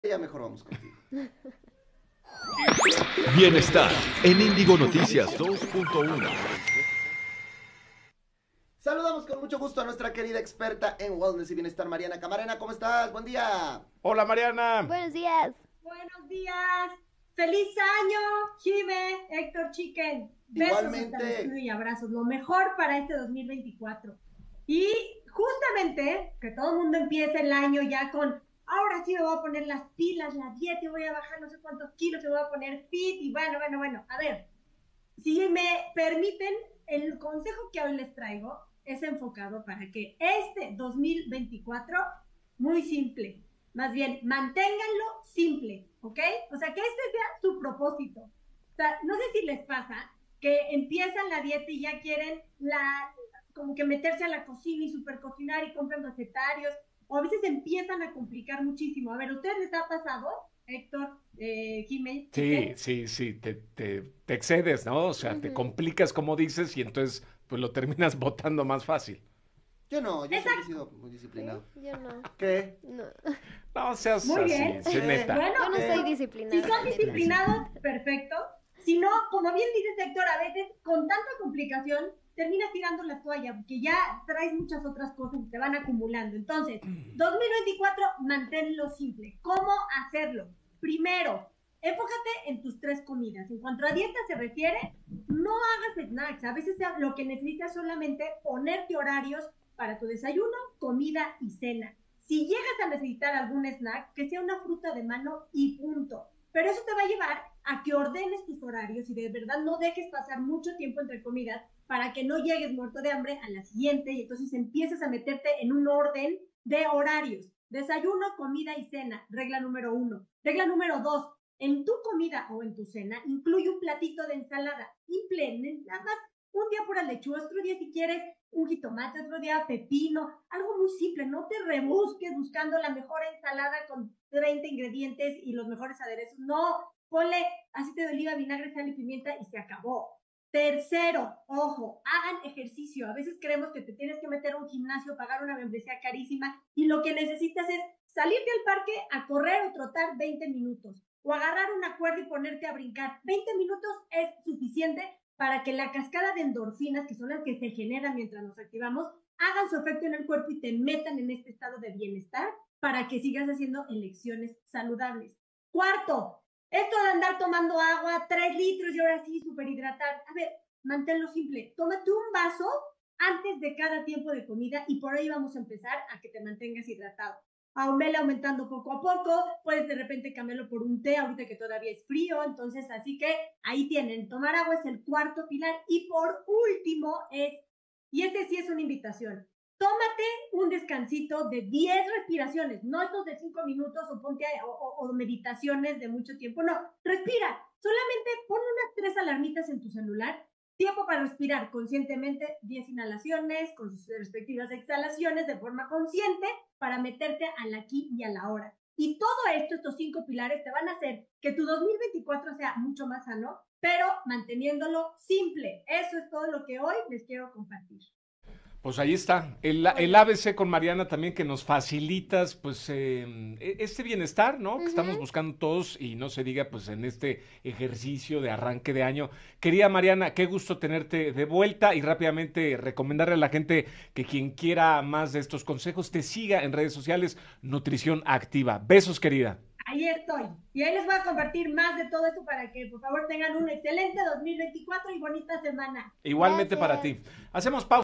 Descubre cómo mantenerte en forma este 2024 I Reporte Indigo
¿Quieres iniciar el año comiendo saludable? Conoce junto a Mariana Camarena algunos consejos que te ayudarán a comenzar el año de la mejor manera.